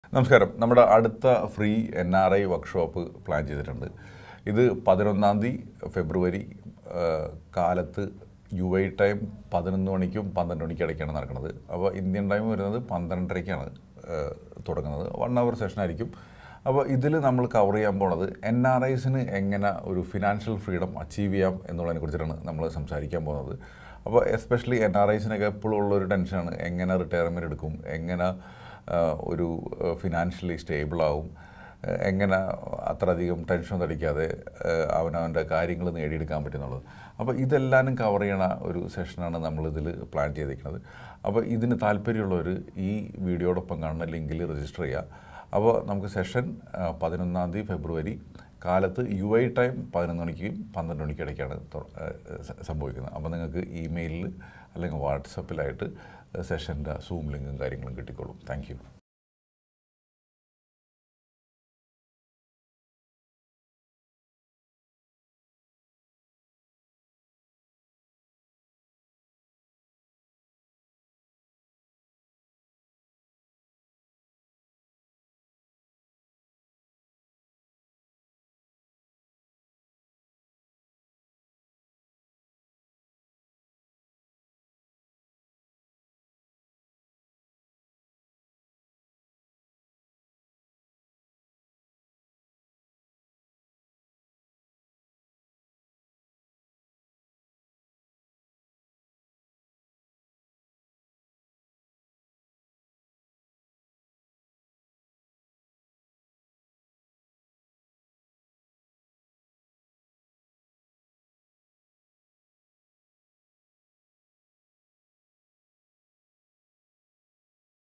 HOW TO ACHIEVE FINANCIAL FREEDOM FOR NRIS | FREE WORKSHOP
Nikhil Gopalakrishnan is inviting you to a scheduled Zoom meeting.
Topic: HOW TO ACHIEVE FINANCIAL FREEDOM FOR NRIS
Time: Feb 11, 2023 11:00 AM (UAE Time)